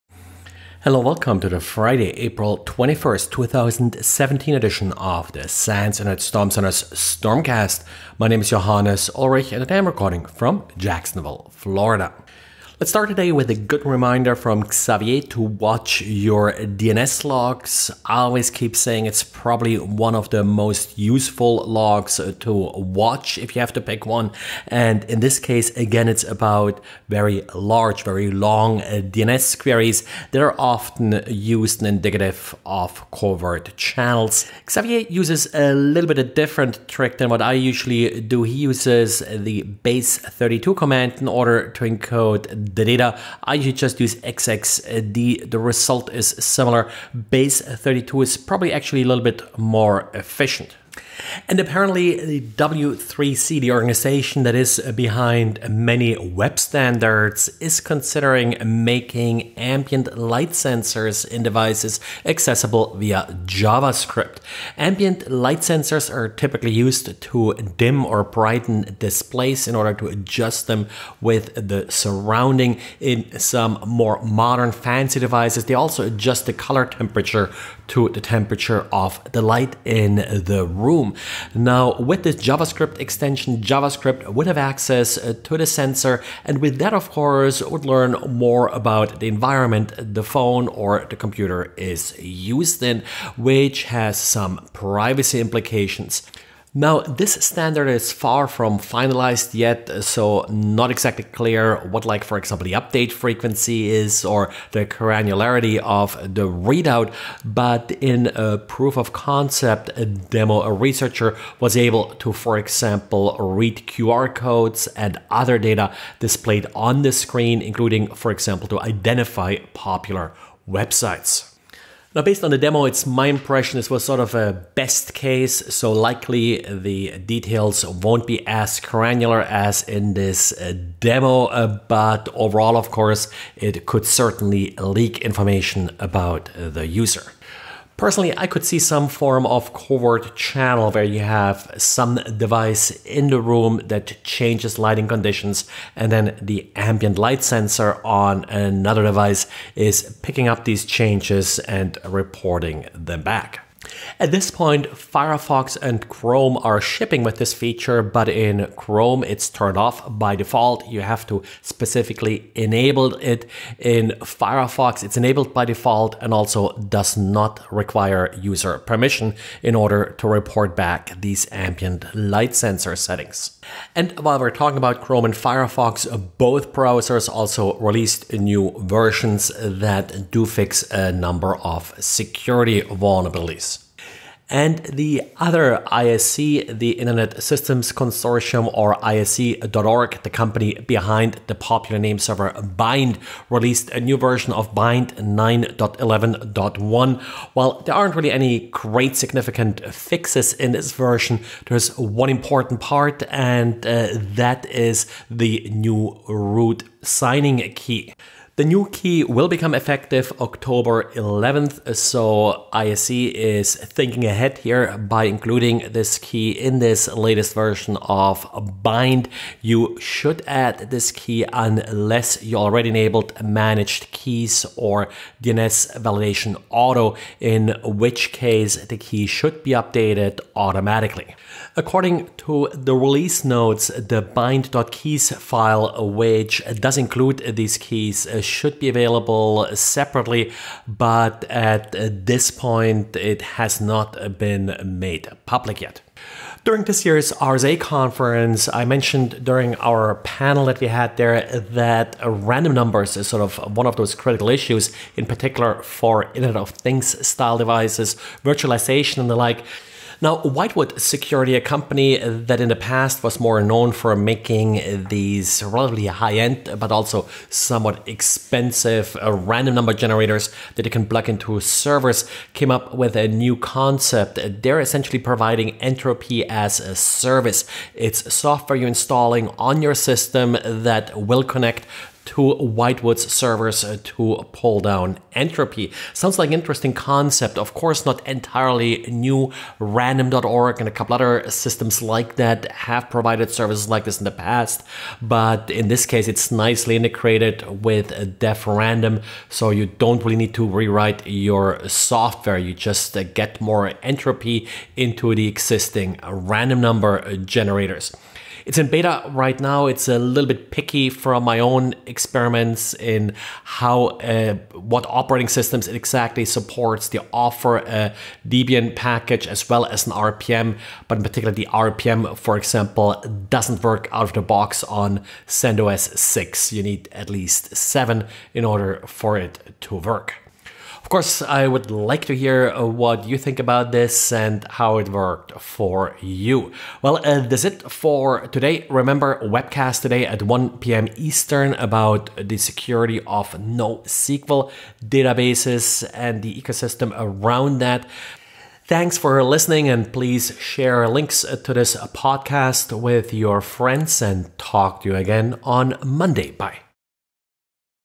Network Security News Summary for Friday April 21 2017
DNS Covert Channels; Ambient Light Sensors;